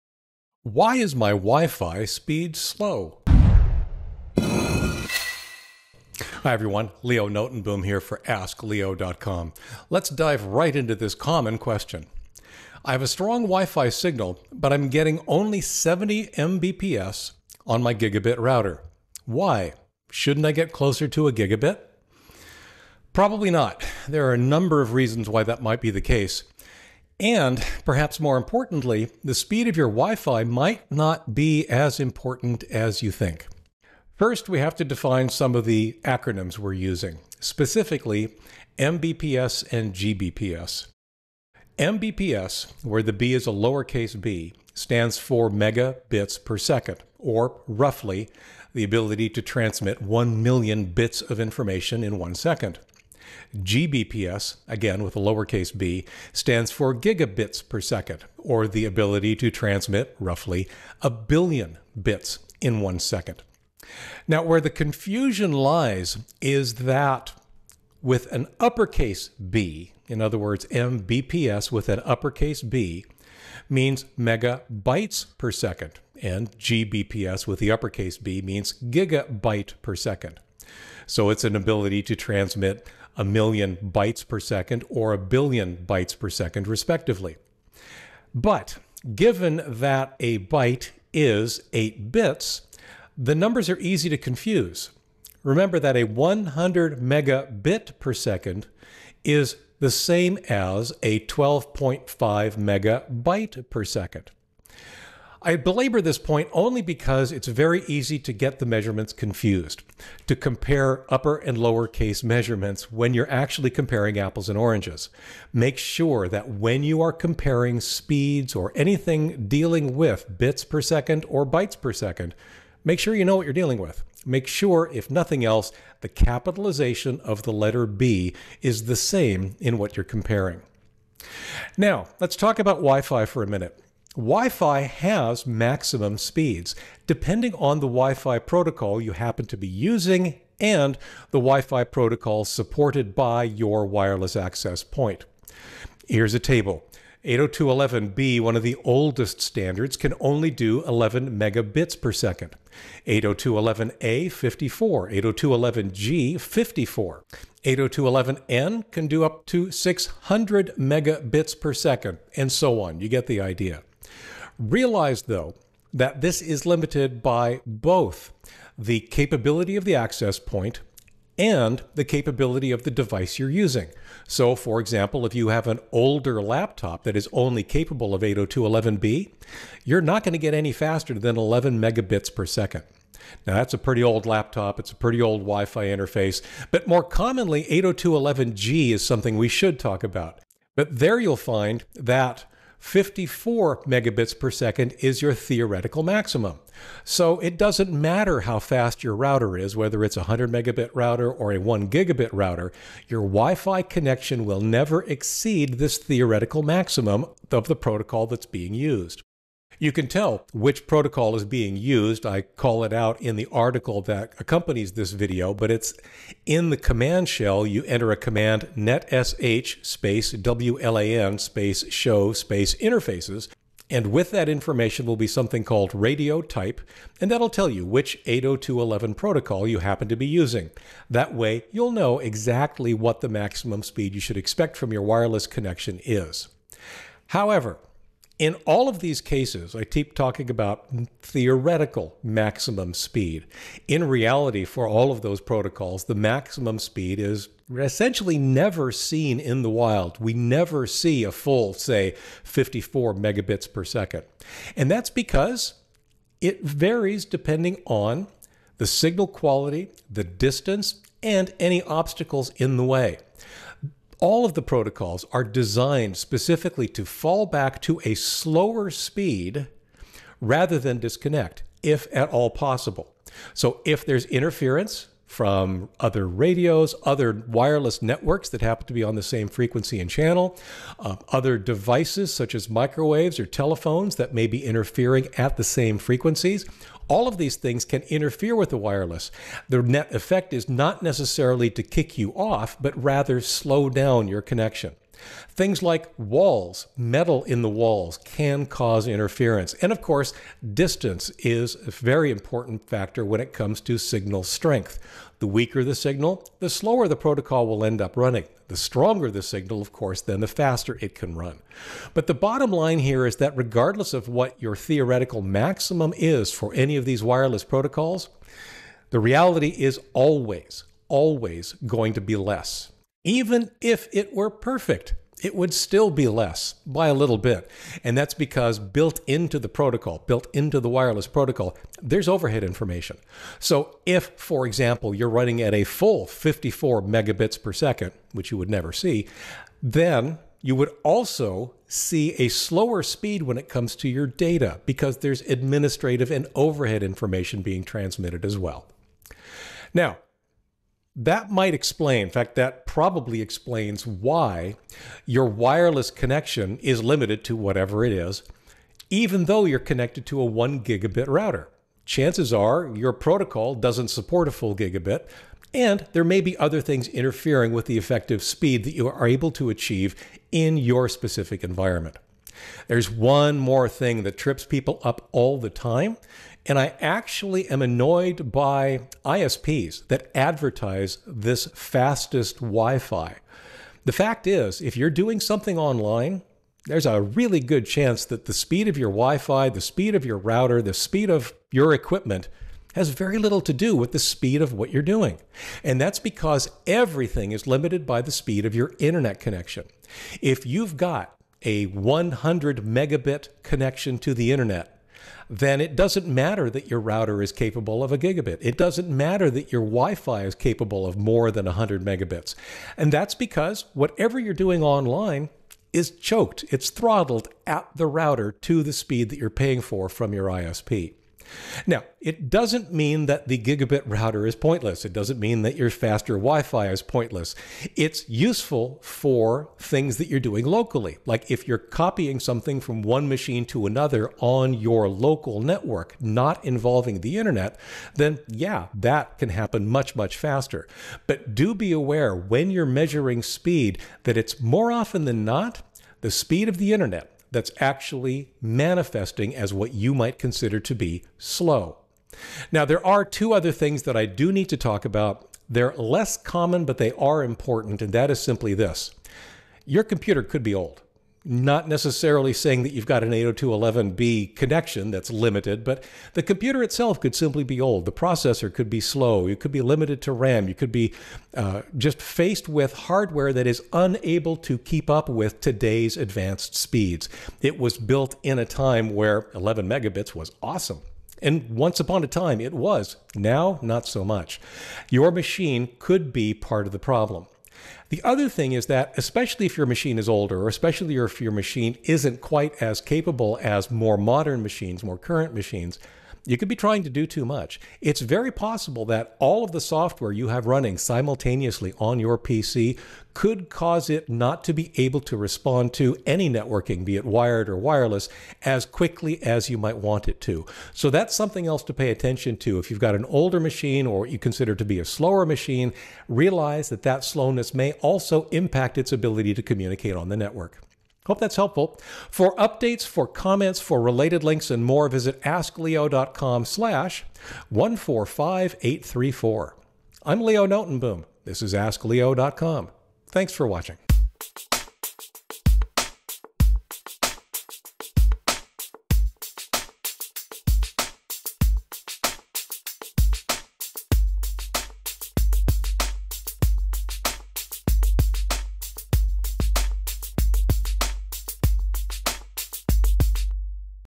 Why is My Wi-Fi Speed Slow?
⏰ Wi-Fi speed can be slow for many different reasons. Slow Wi-Fi can also be a symptom of speed constraints elsewhere.

Wi-Fi speeds can be impacted by the Wi-Fi protocols used, interference from other devices, and more. In addition, other items like slow internet connections and slow or overworked computers can also manifest as slow Wi-Fi.

Chapters
0:00 Why is my wi-fi speed slow
0:35 Mbps and Gbps (and MBps and GBps)
2:10 Wi-Fi speeds and protocols
2:50 Limits of your device
3:35 Find your protocol
4:00 Quality, distance & interference
5:22 Theory versus reality
6:10 Don’t forget the internet
7:40 Your computer